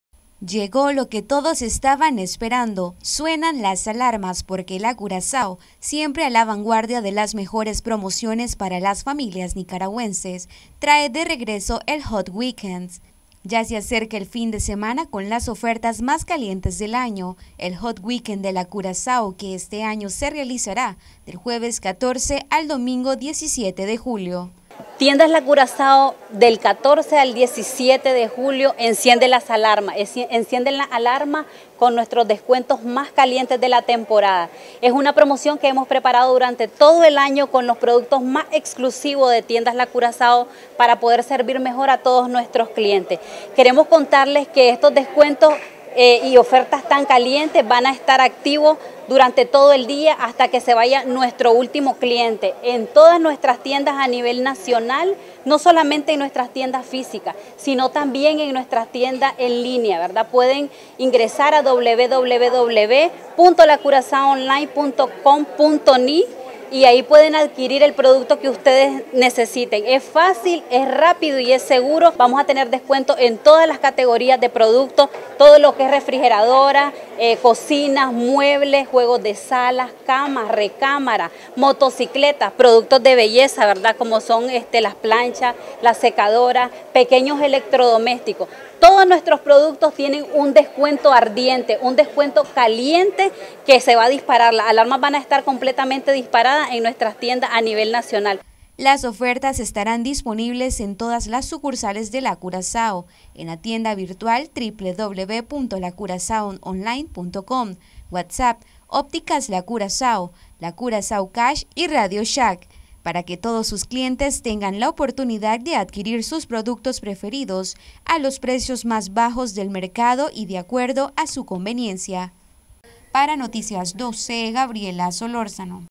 Regresó el Hot Weekend de La Curacao con las ofertas más calientes del año
Noticias12Nicaragua Curacao realiza por séptimo año consecutivo el Hot Weekend donde podrán encontrar productos a super bajos precios.